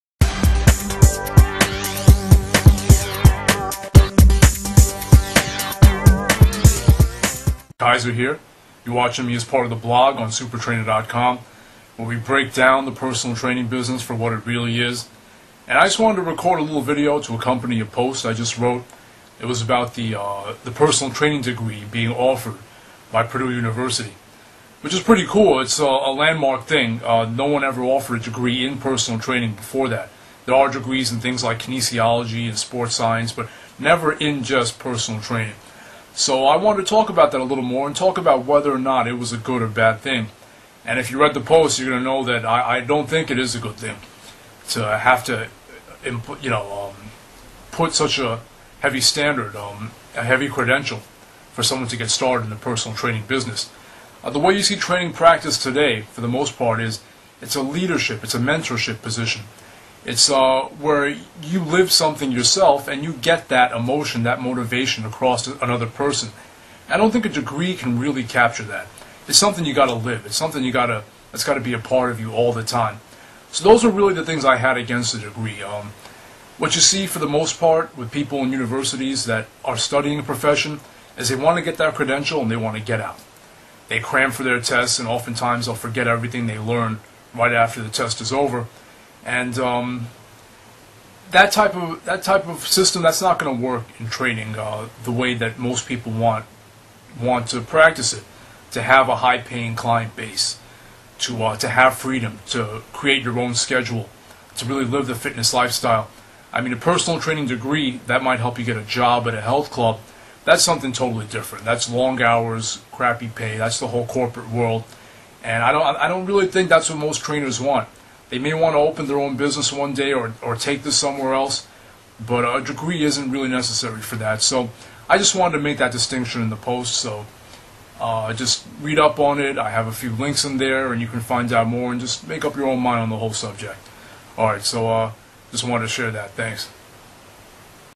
Fitness Marketing - PERSONAL TRAINING DEGREE EXPOSED!!! Intro for SUPER-TRAINER
Kaiser exposes the Degree in Personal Training offered by universities. Watch the video and know why he thinks this is not a good thing.